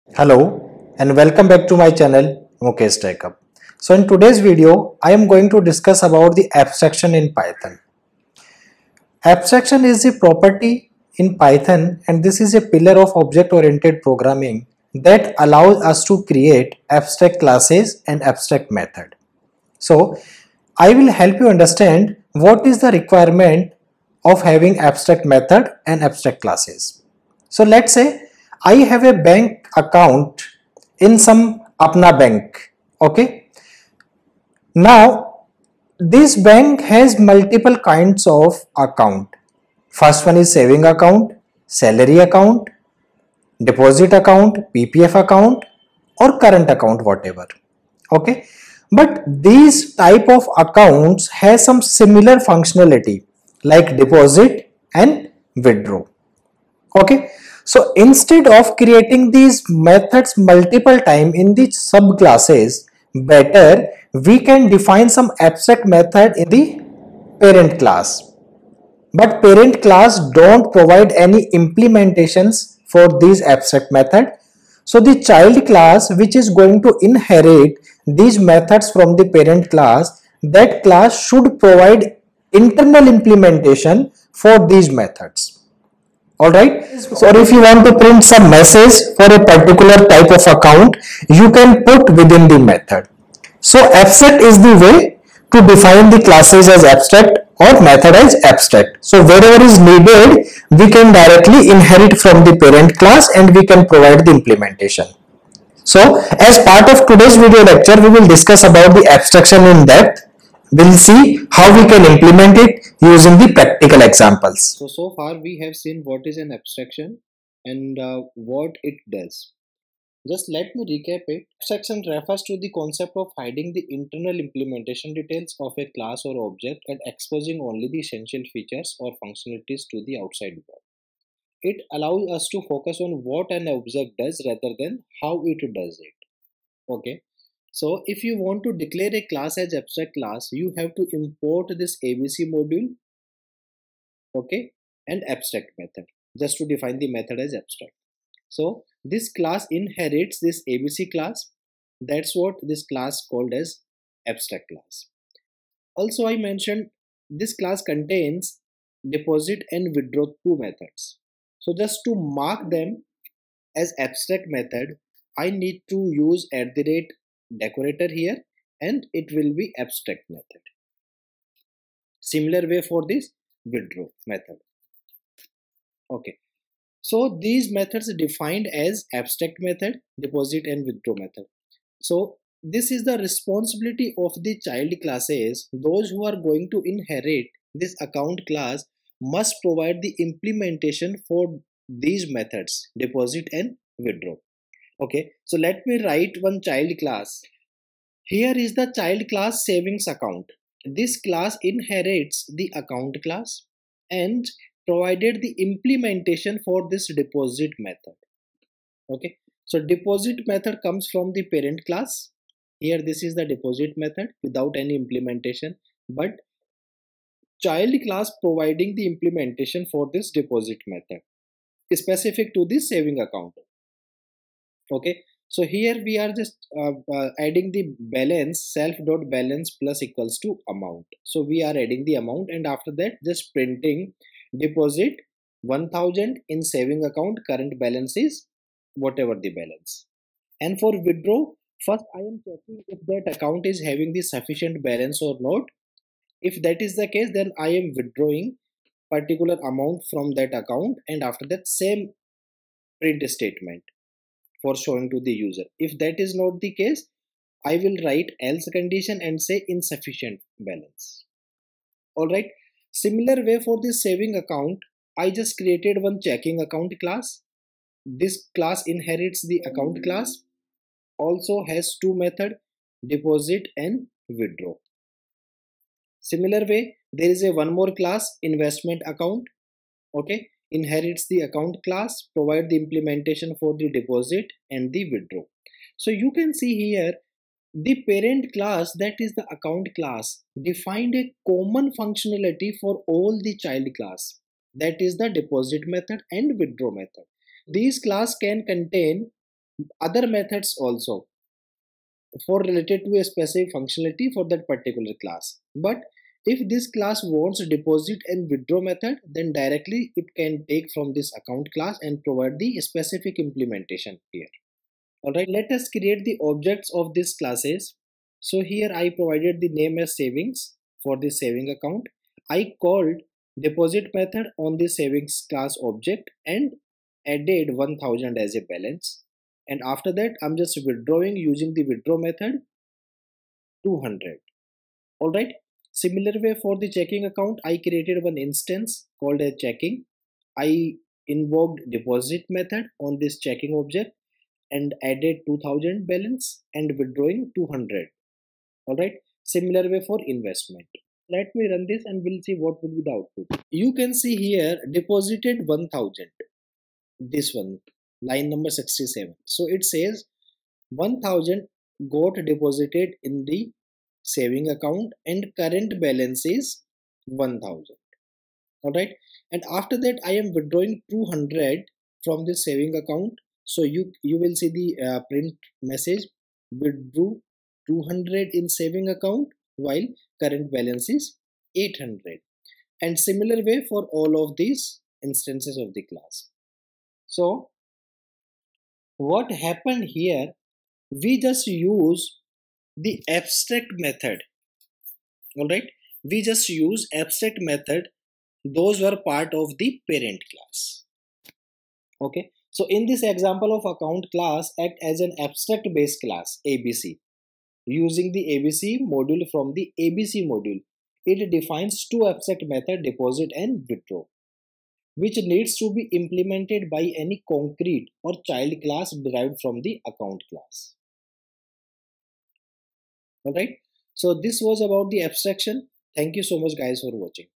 Python Tutorial | What is an Abstraction? | Abstract method
🎥 Get ready to dive into the fascinating world of Python and discover the power of abstraction! 🐍✨ In this highly anticipated YouTube video, we'll unveil the secrets behind abstraction and how it revolutionizes coding. 💡 From beginner-friendly explanations to mind-blowing examples, this video is set to captivate Python enthusiasts and curious learners alike. 🌟 Brace yourself for an epic exploration of abstraction in Python that will leave you craving for more coding adventures! 🚀 Get ready to level up your programming skills and unleash your creative genius as we unravel the wonders of abstraction in this viral YouTube sensation! 🔥